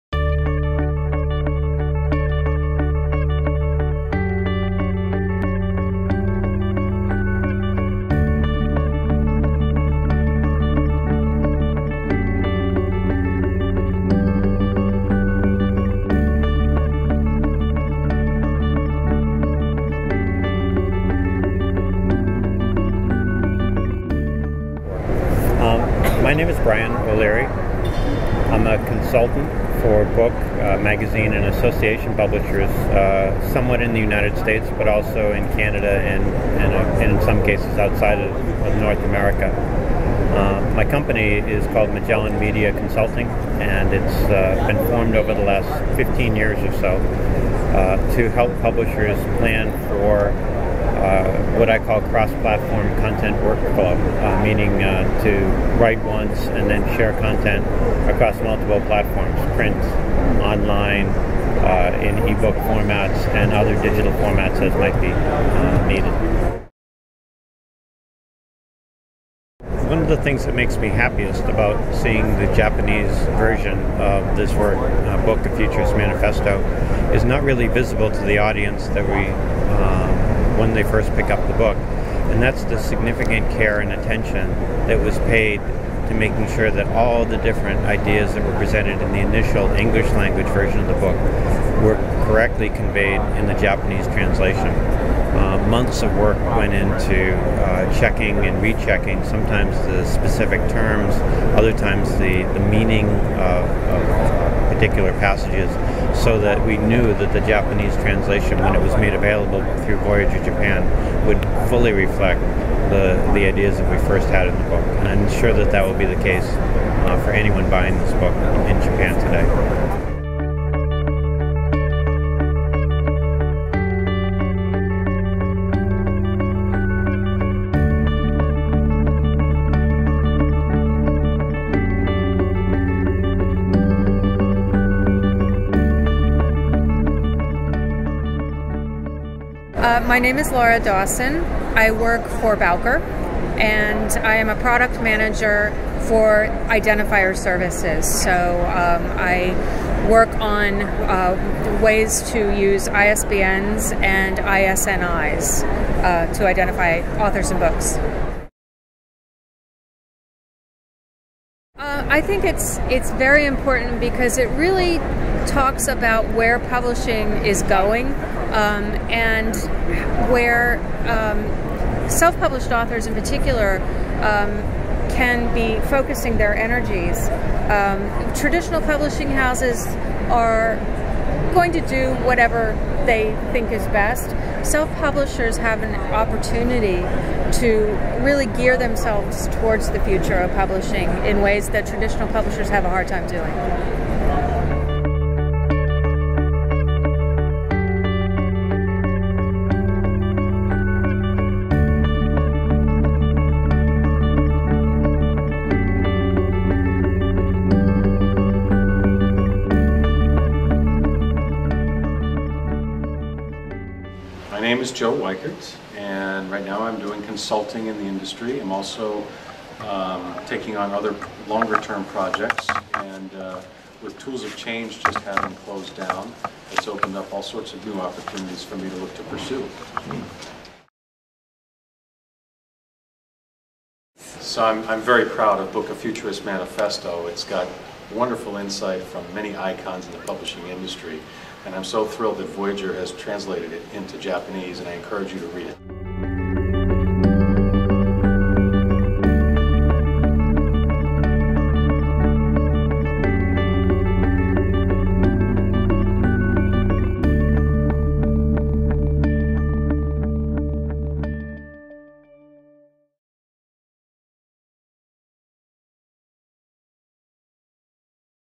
国際電子出版エキスポPV：Manifesto vol.1
2013/7/3〜7/5開催『国際電子出版EXPO2013』のボイジャーブースで上映したプロモPVをご紹介！

『マニフェスト  本の未来』の編集者、著者たちから日本語翻訳版発行にあたりコメントをいただきました！！日本の読者へのメッセージをお楽しみください！
（撮影：IDPF Digital Book May 29-30, 2013, from NY USA）

▷ブライアン・オレアリ氏（マゼランメディアパートナーズ）
『マニフェスト  本の未来』編集者
 第1章「コンテナではなく、コンテキスト」著者
 第6章「デジタルワークフロー向けツール」著者

▷ローラ・ドーソン氏（バウカー）
 第4章「メタデータについて語る時に我々の語ること」著者

▷ジョー・ワイカート氏
（出版業界コンサルタント／O'REILLY TOC(Tools of Change）関係者）

『マニフェスト 本の未来』印刷版＋電子版、好評発売中！！
書籍のデジタル化のツール開発に携る総勢29人の執筆者たちのエッセイ集を翻訳した内­­容（2012年・O'REILLY社刊「Book:A Futurist's Manifesto」）。

▶「マニフェスト 本の未来」
最新情報（書評、イベント等）はコチラ